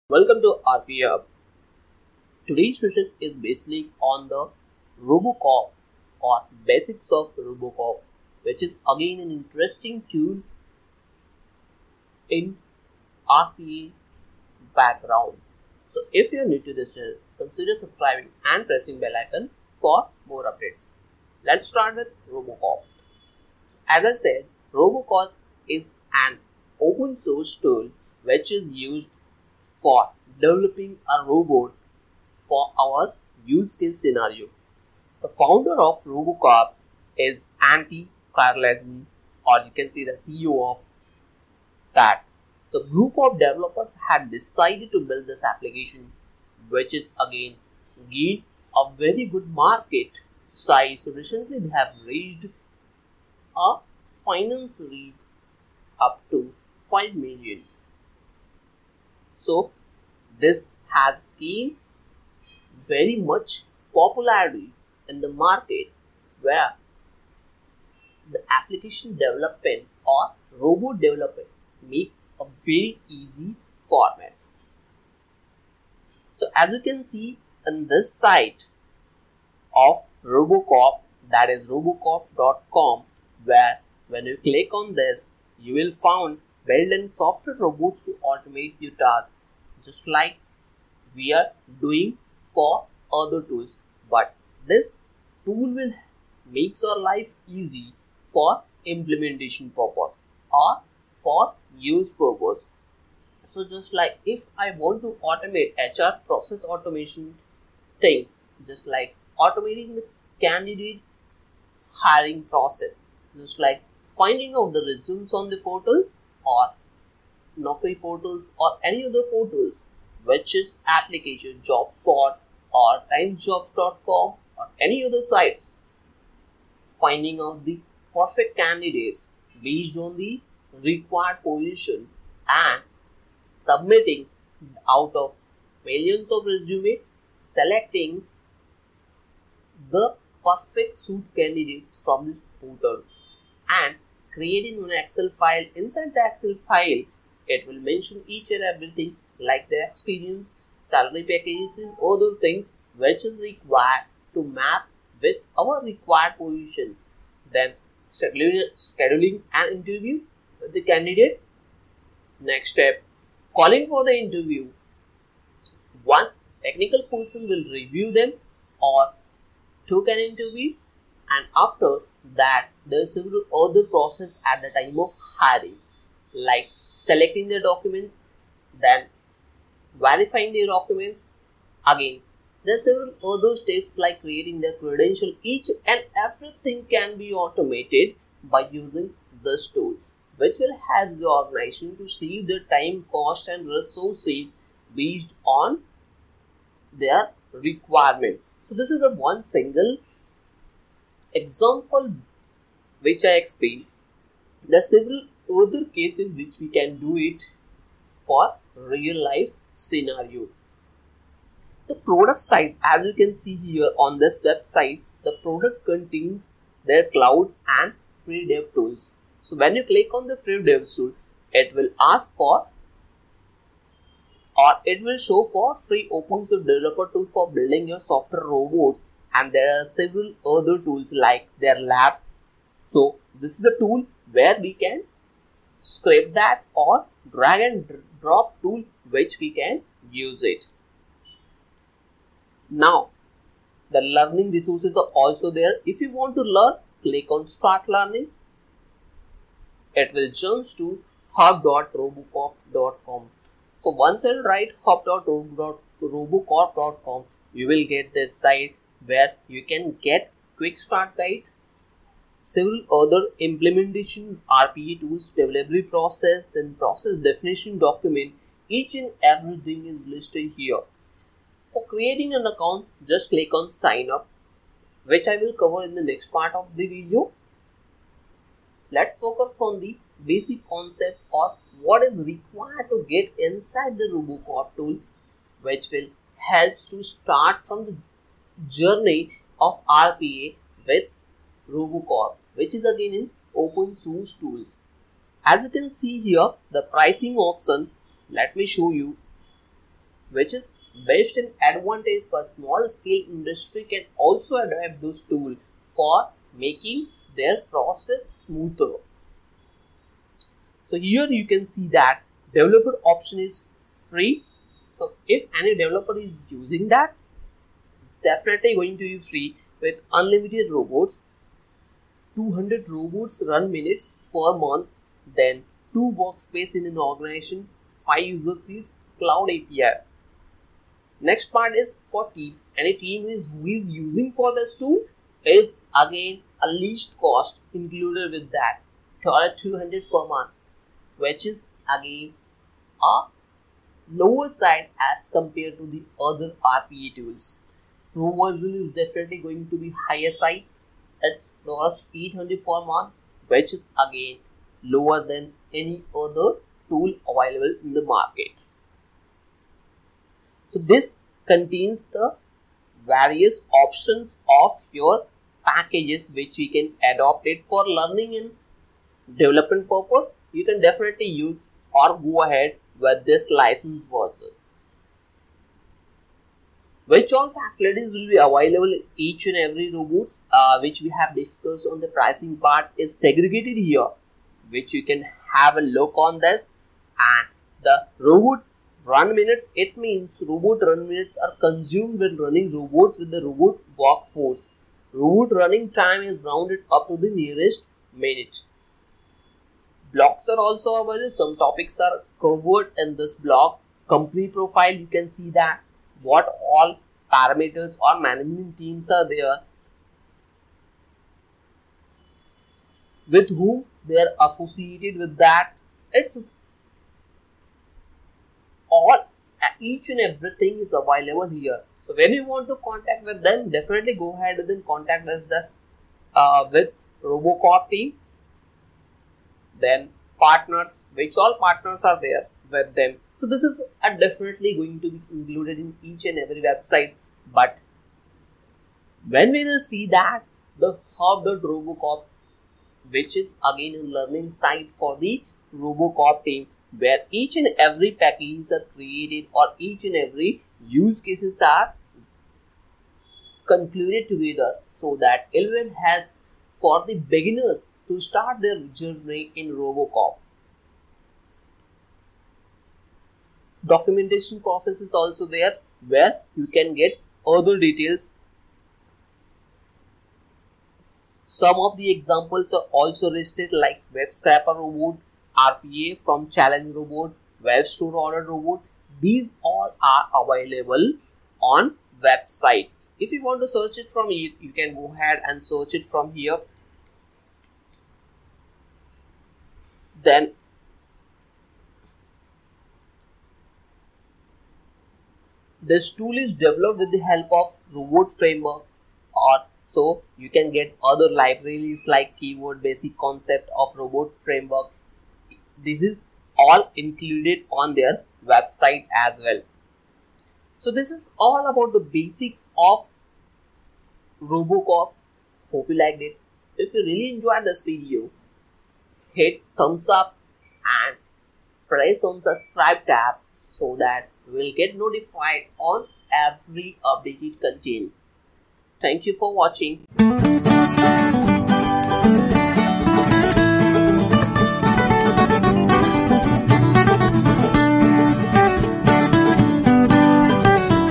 Introduction of Robocorp | Open Source RPA Tool | Robocorp Overview
Robocorp was founded on the realization that robotic process automation is fundamentally a developer domain. That's why team build their products keeping developers in mind. But developers are people, too. So their design approach is to use syntax and language that is easy to write and is easy to read.

Watch complete Session for more detail.

Get Updated with RPA Hub!!